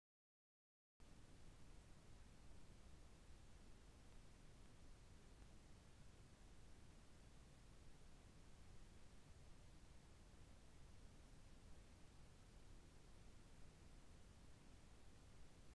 Partridge drumming
Video of a Maine grouse (partridge) standing on a log and drumming